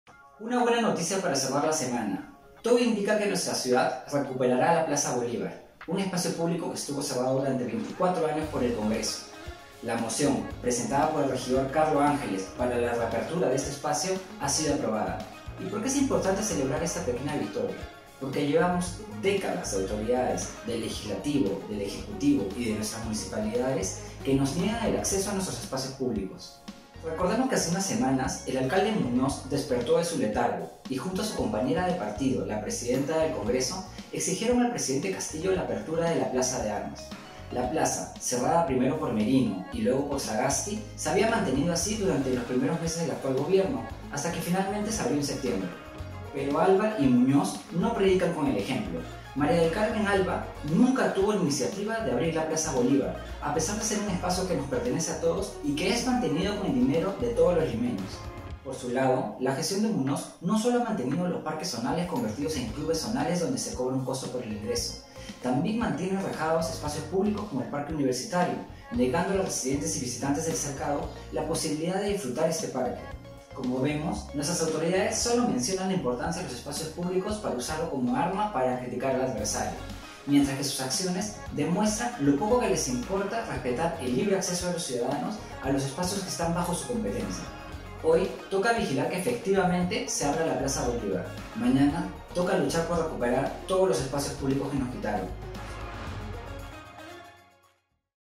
Luego de 24 años, ciudadanos podrán ingresar a la plaza Bolívar
Ya no será cochera del Congreso. Municipalidad de Lima empezó a gestionar la apertura de la plaza Bolívar y los jirones aledaños, por ser espacios públicos.